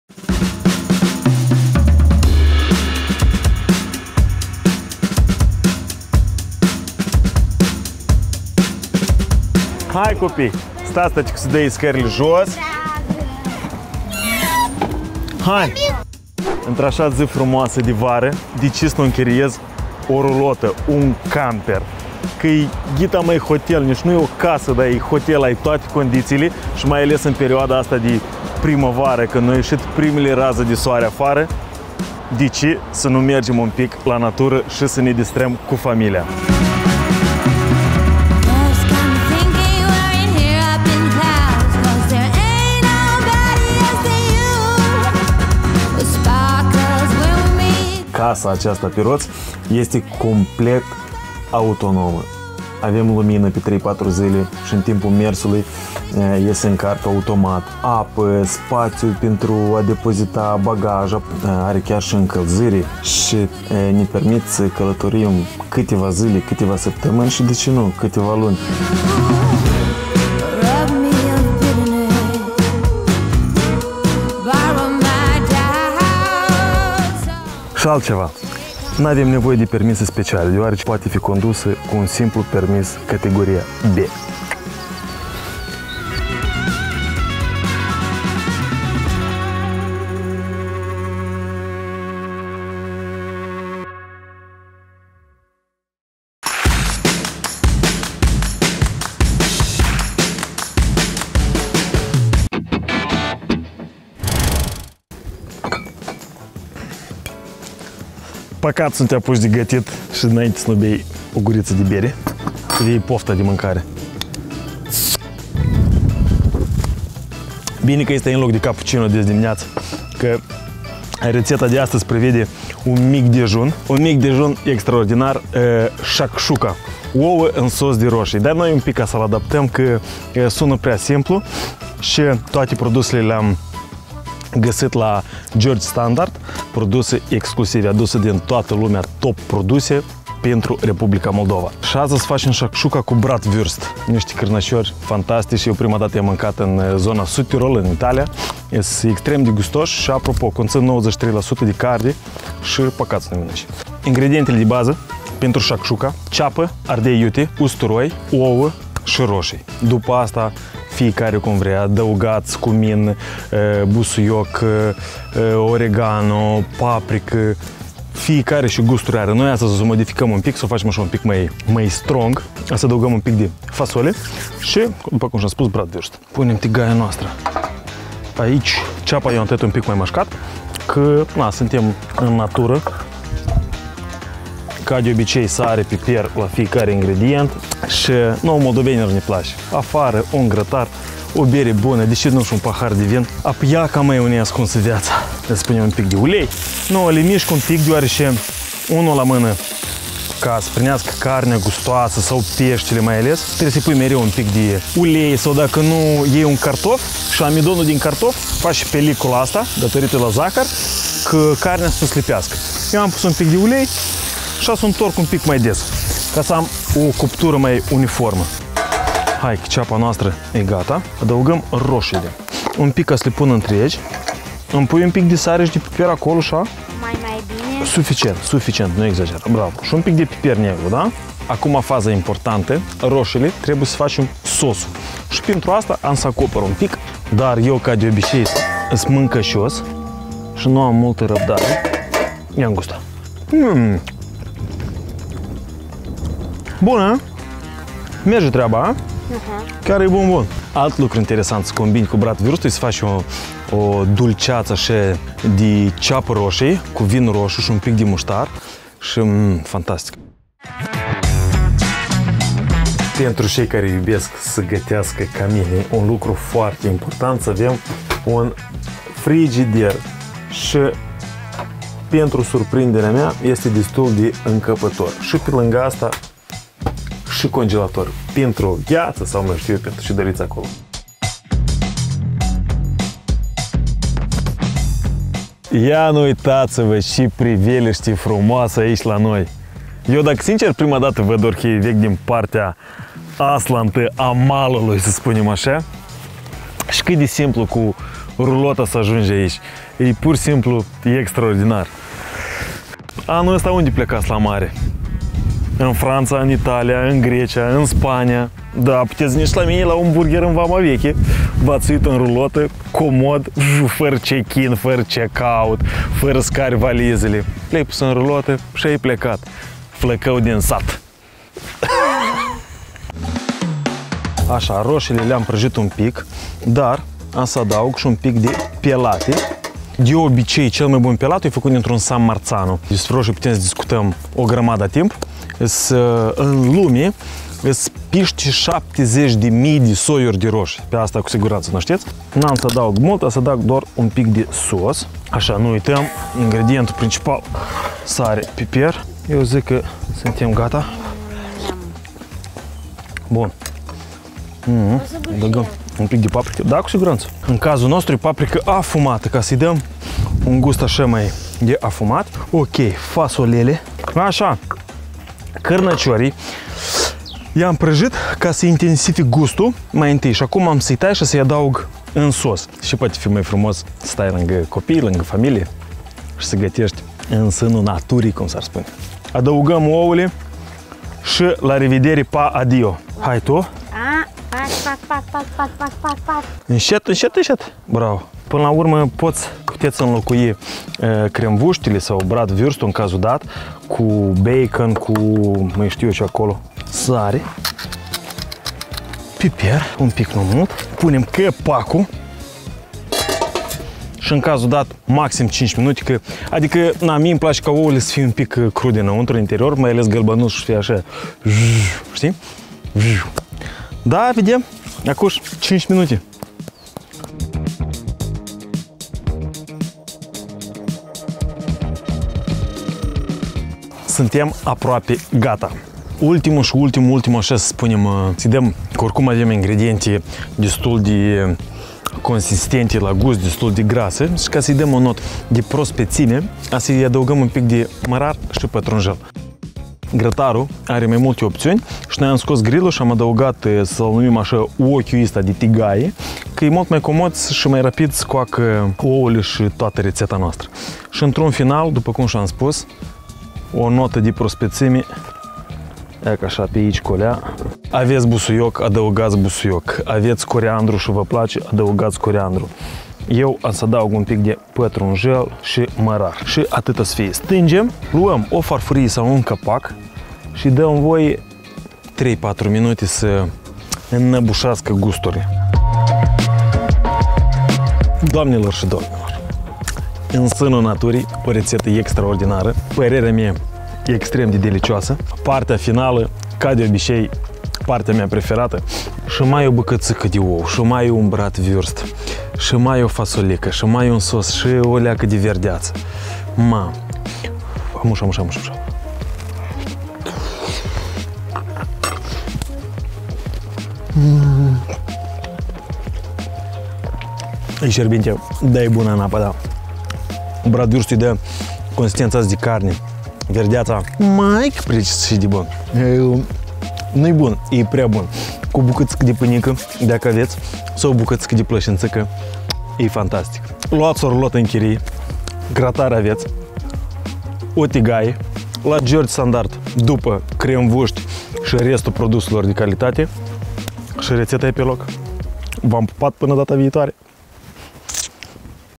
Ediție specială! Mic dejun la Orheiul Vechi!
Dăm start sărbătorilor de mai cu o rețetă simplă, ingrediente delicioase, în aer liber cu familia- de-o dragoste!

INGREDIENTE
Cârnăciori wurst
Ouă
Fasole conservate
Roșii cherry
Roșii pelatti
Ardei iute
Ceapă
Mărar
Pătrunjel
Ulei
Sare
Piper
Paprika

Încingem grătarul și punem cârnăciorii la prăjit cu puțin ulei.
În tigaia încinsă punem ceapa tocată mai mășcat. Important- adăugăm sare și piper la fiece ingredient nou.
Peste ceapă adăugăm roșiile cherry întregi și acoperim cu capacul.
Puțin sos pelatti pentru gust și paprika afumată, după care adăugăm cârnăciorii tăiați și fasolea.
Stricăm câteva ouă, condimentăm și punem capacul pentru maxim 5 minute.
Pentru un plus de prospețime, adăugăm mărarul și pătrunjelul tocat mărunt.
Poftă bună!